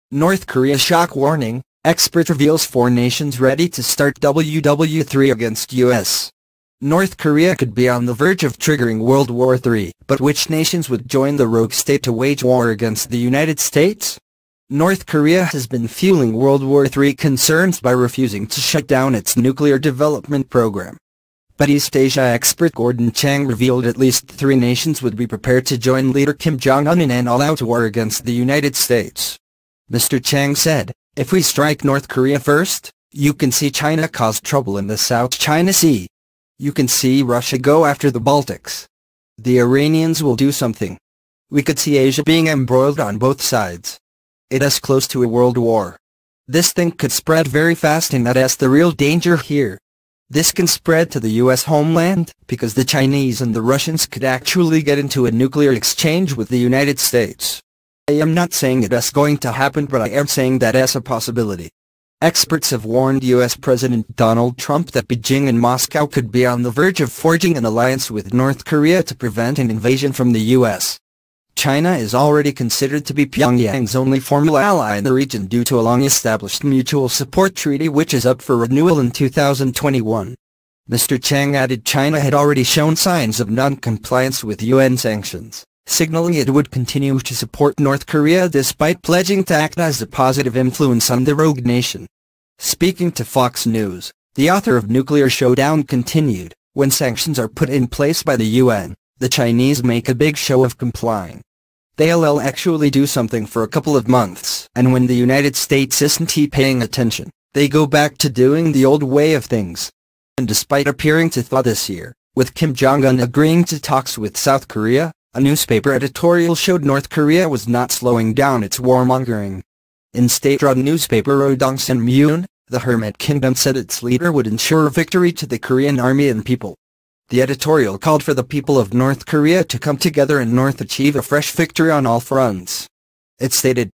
North Korea SHOCK warning Expert reveals four nations ready to start WW3 against US
NORTH KOREA could be on the verge of triggering World War 3 – but which nations would join the rogue state to wage war against the United States? North Korea has been fueling World War 3 concerns by refusing to shut down its nuclear development programme. But East Asia expert Gordon Chang revealed at least three nations would be prepared to join leader Kim Jong-un in an all-out war against the United States. Mr Chang said: "If we strike North Korea first, you can see China cause trouble in the South China Sea. You can see Russia go after the Baltics. The Iranians will do something. "We could see Asia being embroiled on both sides. It’s close to a world war. This thing could spread very fast and that’s the real danger here. "This can spread to the US homeland because the Chinese and the Russians could actually get into a nuclear exchange with the United States. I’m not saying it’s going to happen but I’m saying that’s a possibility." Experts have warned US President Donald Trump that Bejing and Moscow could be on the verge of forging an alliance with North Korea to prevent an invasion from the US. China is already considered to be Pyongyang's only formal ally in the region due to a long-established mutual support treaty which is up for renewal in 2021. Mr Chang added China had already shown signs of non-compliance with UN sanctions, signalling it would continue to support North Korea despite pledging to act as a positive influence on the rogue nation. Speaking to Fox News, the author of 'Nuclear Showdown' continued: "When sanctions are put in place by the UN, the Chinese make a big show of complying. "They’ll actually do something for a couple of months and when the United States isn’t paying attention, they go back to doing the old way of things." And despite appearing to thaw this year, with Kim Jong-un agreeing to talks with South Korea, a newspaper editorial showed North Korea was not slowing down its warmongering. In state-run newspaper Rodong Sinmun, the hermit kingdom said its leader would ensure “victory to the Korean army and people”. The editorial called for the people of North Korea to come together and North “achieve a fresh victory on all fronts”. It stated: “No force on the earth can block the vigorous advance of Korea as it is under the outstanding and tested guidance of the Workers' Party of Korea which leads our style of socialism to victory." Talks between the Korean neighbours are set to be held on January 9, despite warnings from the US it is a bluff and Pyongyang’s nuclear ambitions show no sign of slowing.
The negotiations will be the first in two years and will be held at the border truce village of Panmunjom.